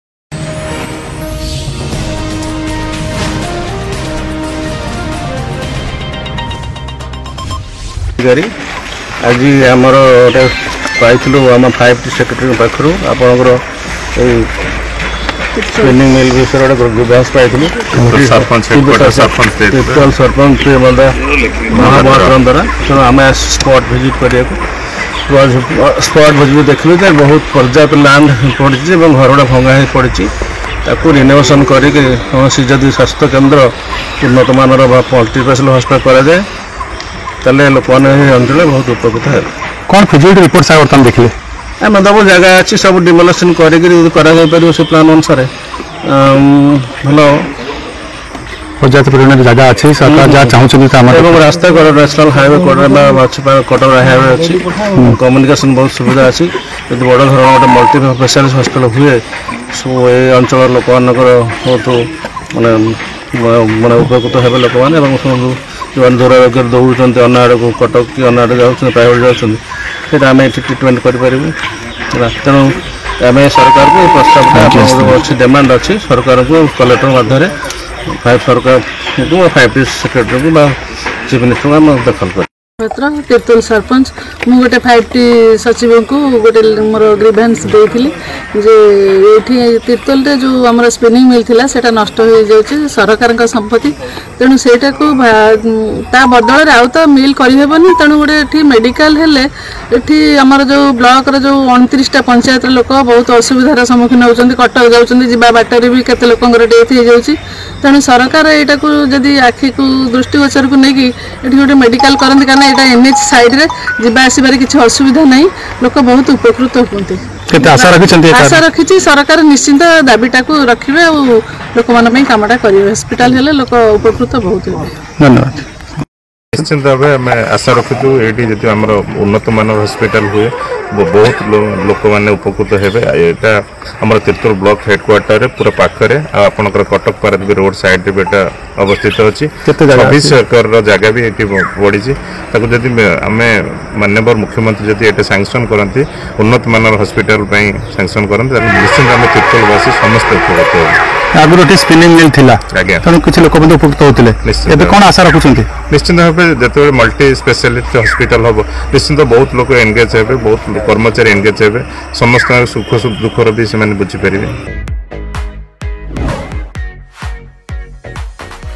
Tirtol: District officials have decided to build a super-facility hospital
About Channel:
ଚ୍ୟାନେଲ୍ ବିଷୟରେ.